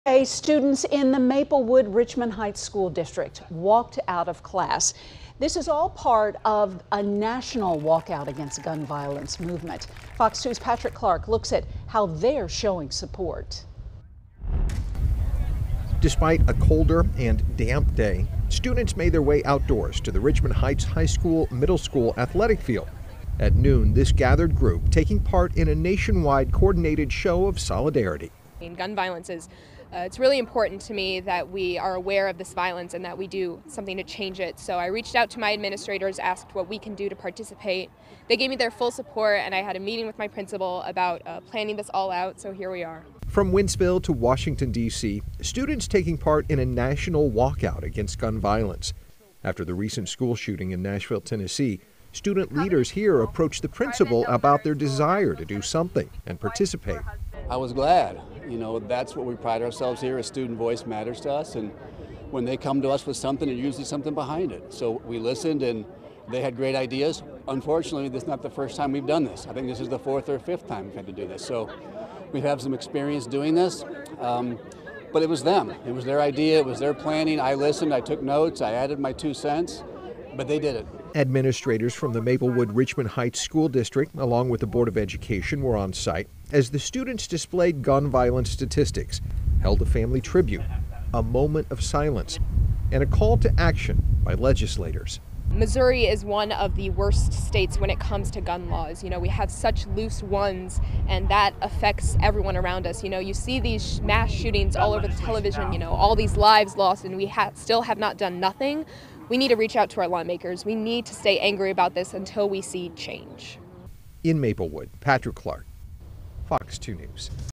St. Louis County high school students walk out of class to show support against gun violence
Despite a colder and damper day, students made their way outdoors to the Maplewood Richmond Heights High School and the middle school athletic field. At noon on Wednesday, a group gathered and took part in a nationwide coordinated show of solidarity.